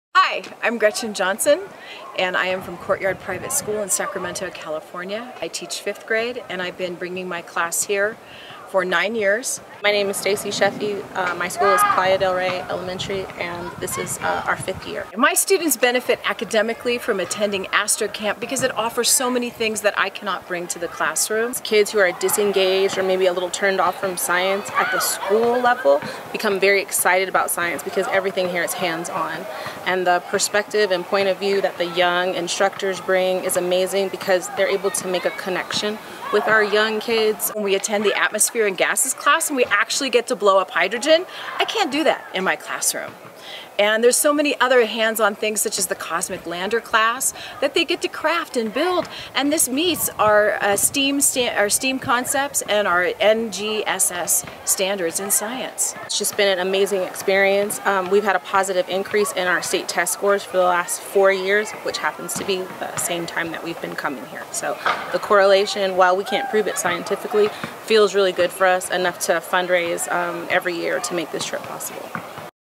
AstroCamp Academic Benefits
We asked a couple of our school year trip leaders why they continue to bring their students to our program.